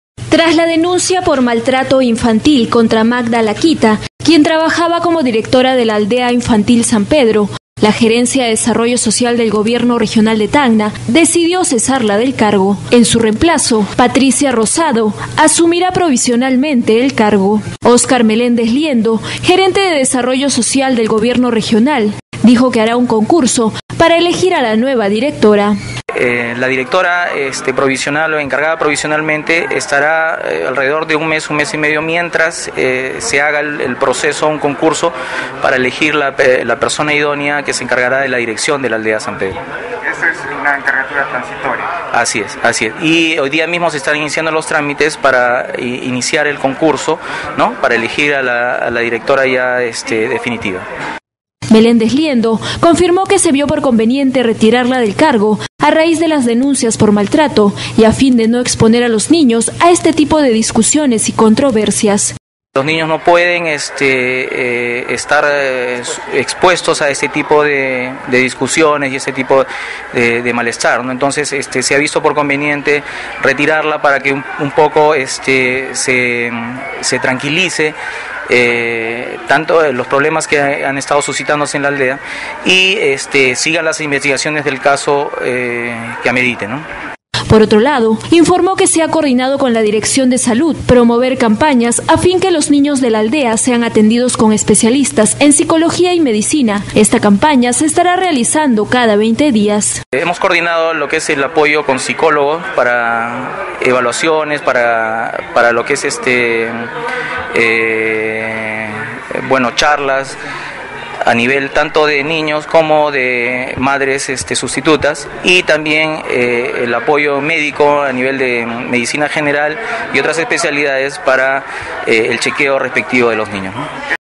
NOMBRAN DIRECTORA PROVISIONAL EN ALDEA SAN PEDRO
Tras la denuncia  por maltrato infantil, contra  Magda laquita, quien se desenvolvía como directora de la Aldea Infantil San Pedro, gerencia de desarrollo social del gobierno regional  de Tacna, decidió cesarla del cargo.  Patricia Rosado, asumirá provisionalmente cargo.